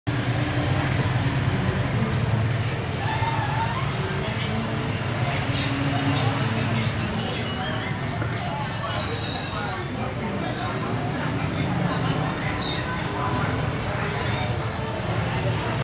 Bà đi chợ. quên mang tiền
bà già rồi đầu óc hay quên lãng, cháu ơi